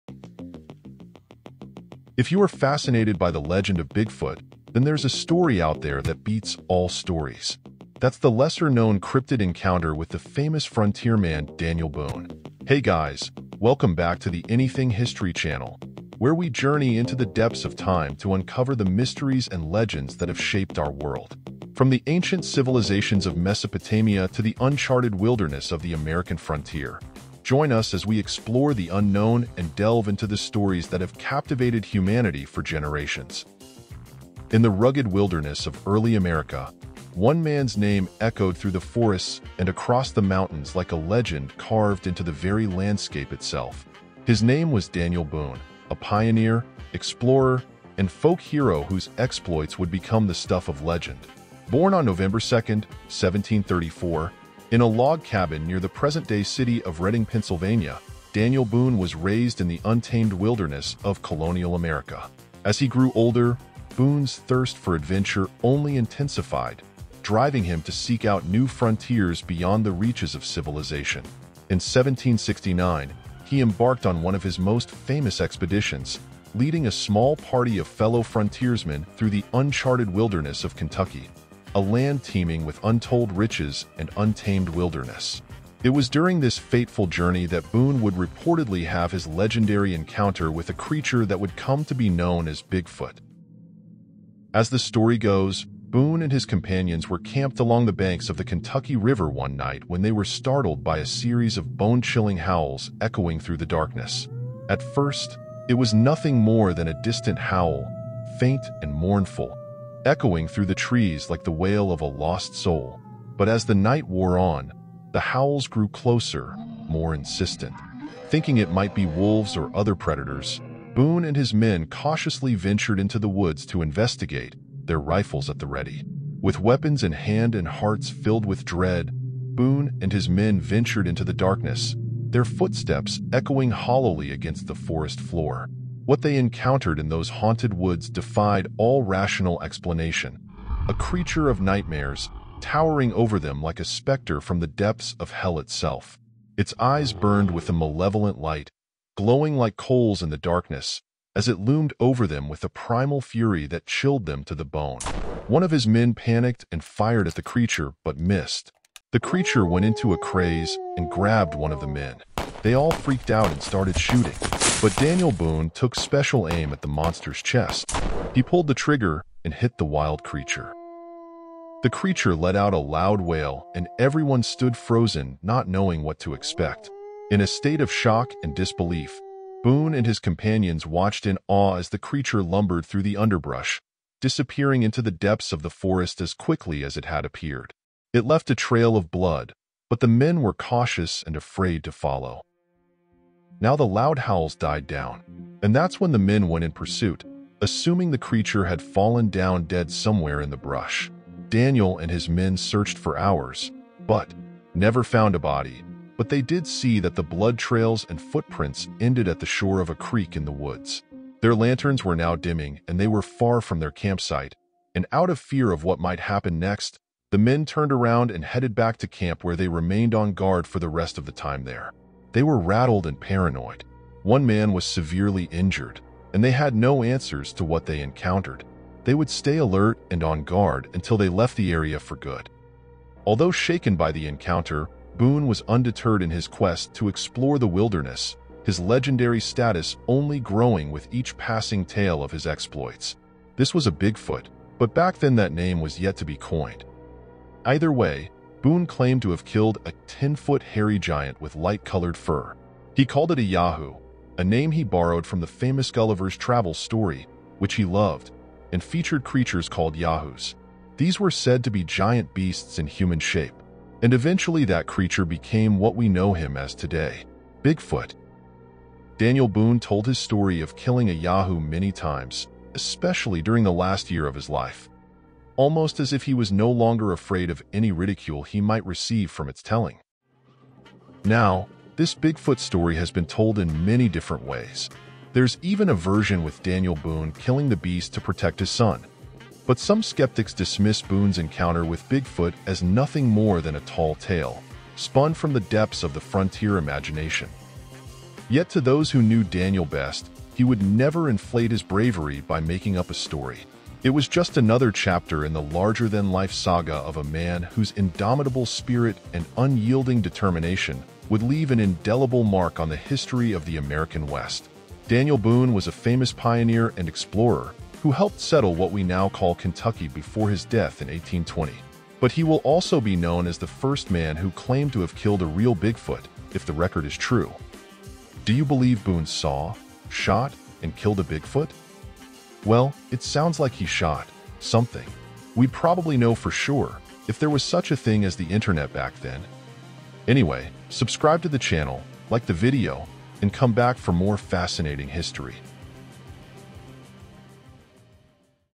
Daniel Boone's Encounter: The Legend of Bigfoot
Join us as we uncover the chilling tale of Daniel Boone's legendary encounter with Bigfoot in the untamed wilderness of early America. In the depths of the Kentucky forest, Boone and his companions found themselves face to face with a creature of nightmares—a towering behemoth with glowing red eyes and shaggy fur. Explore the history, dates, and events surrounding this cryptid legend as we delve into eyewitness accounts and the enduring mystery that continues to haunt the American frontier.